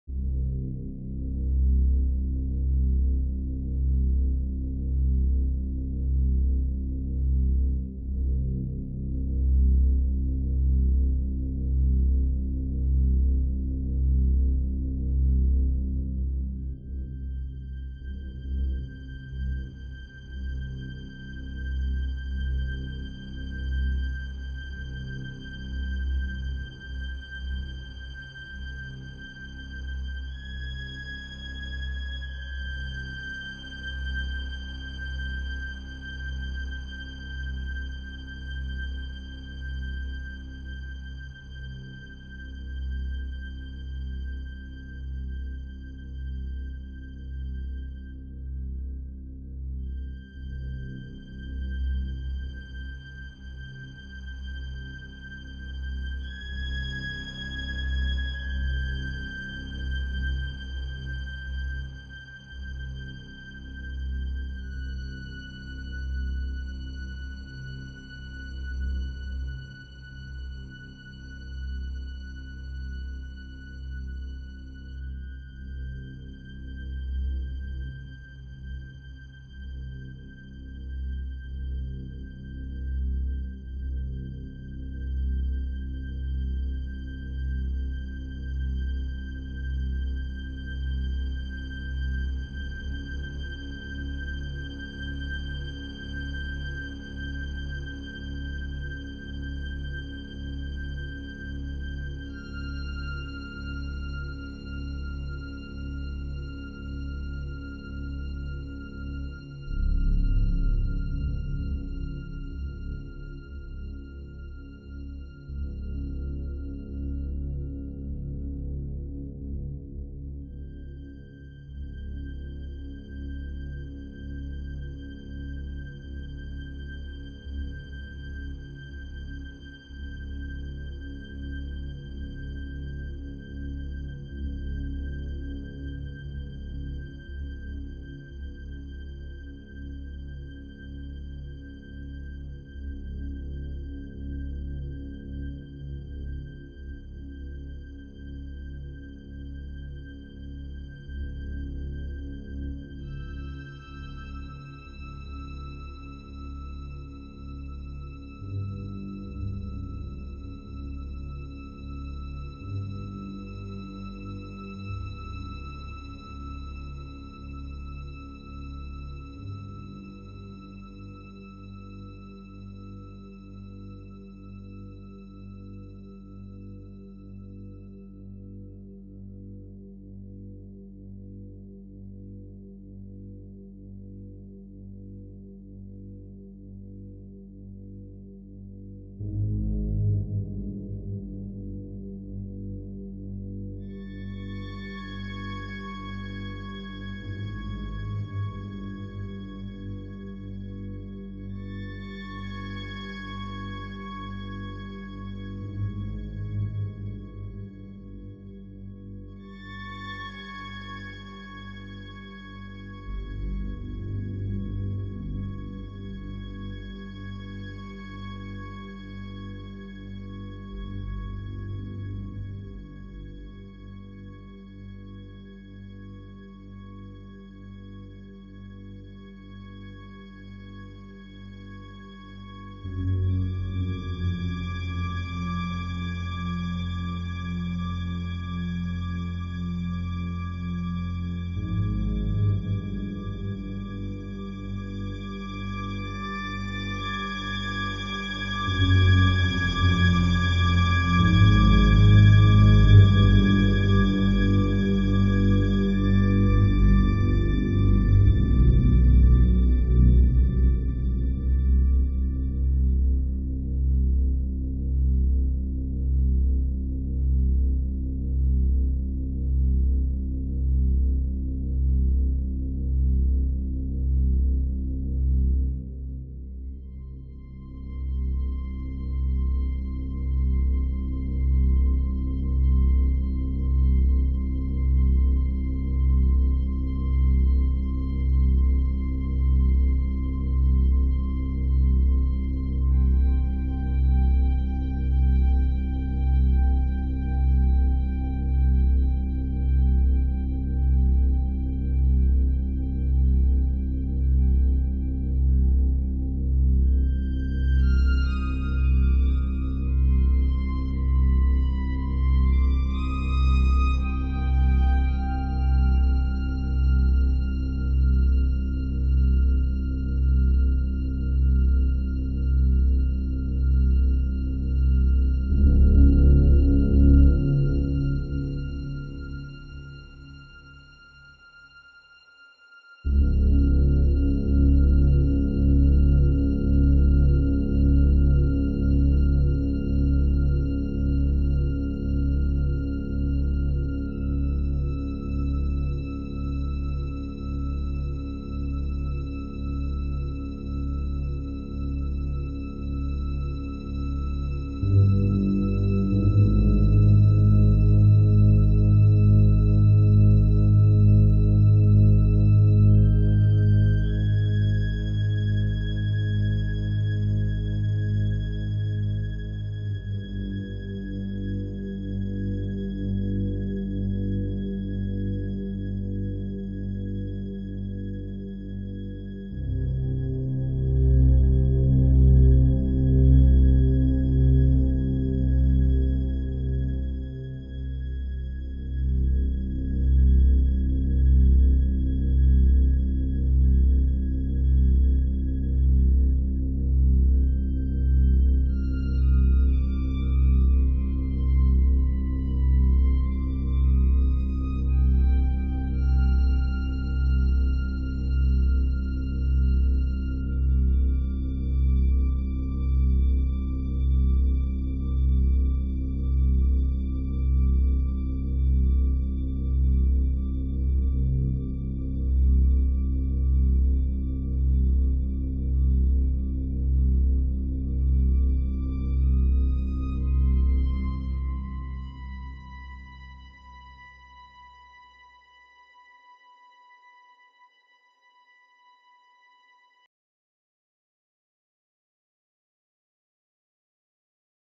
Alaskan Malamute in Ljubljana: Embassies and Faculties
A new video of Liza, the Alaskan Malamute, exploring the Ljubljana. this time we went to see some of the embassies along the Prešeren Street and faculties along the Aškerc Street. More info below.